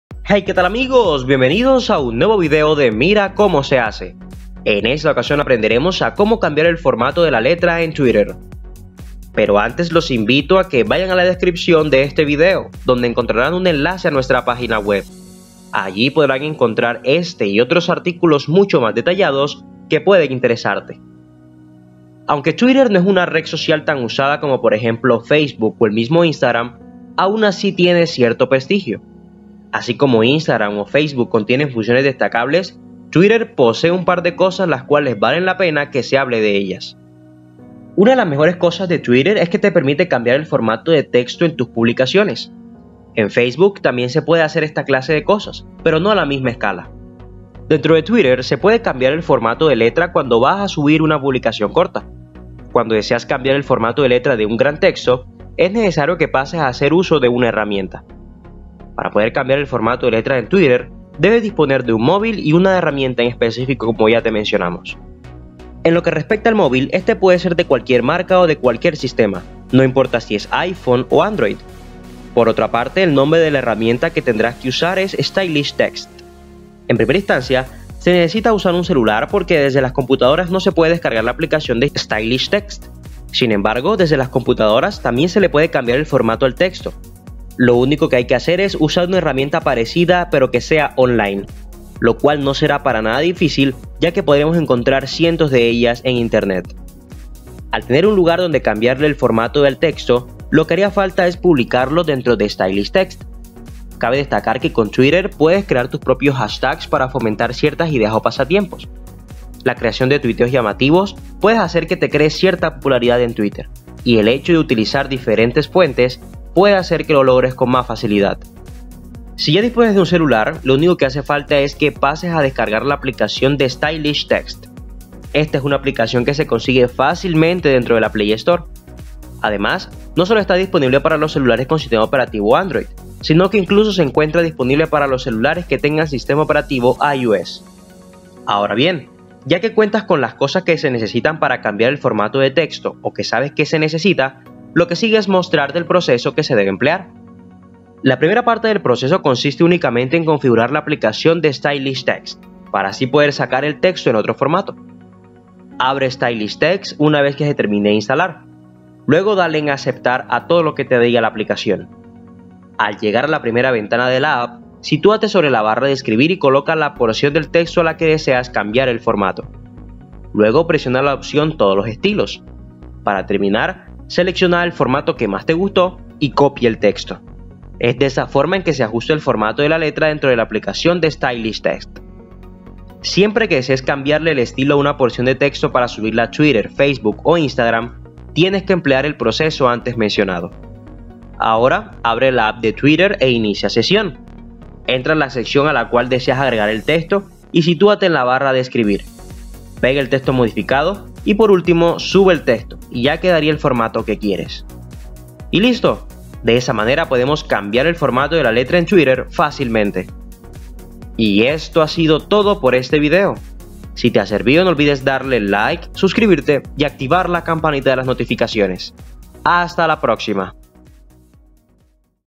¿Cómo Cambiar el Formato de la Letra en Twitter? - Negrita, Cursiva y Subrayado
En este video te mostramos Cómo Cambiar el Formato de la Letra en Twitter. En este canal encontrarás los mejores tutoriales. ¡No te lo pierdas!